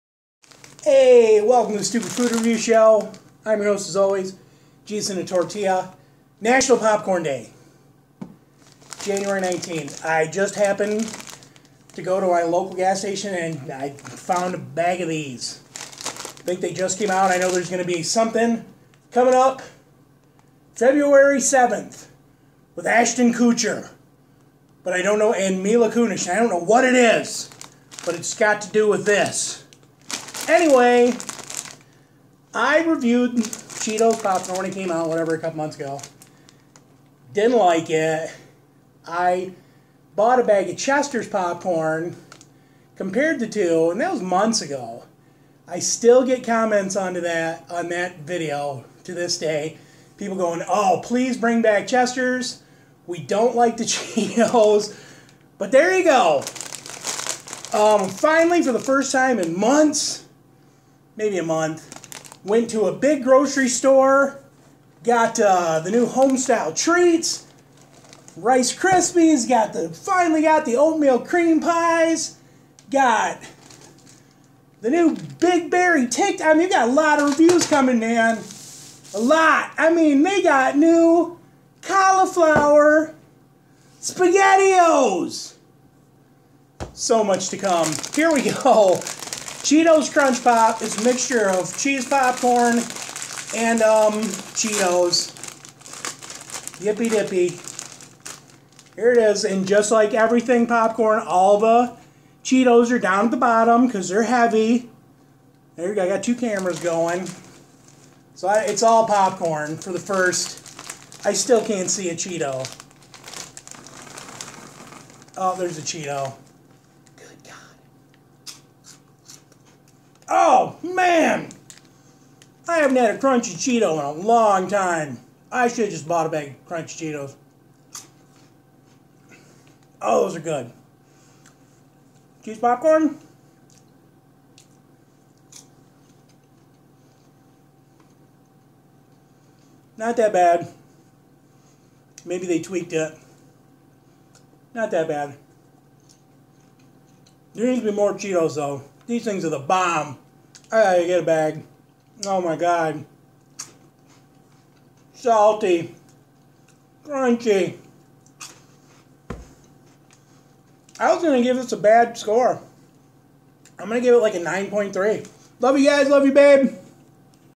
Celebrating National Popcorn Day Jan 19th, With NEW Cheetos Crunch Pop Mix!🐆🌽
New Cheetos Crunch Pop Mix.
fb: Jesus In A Tortilla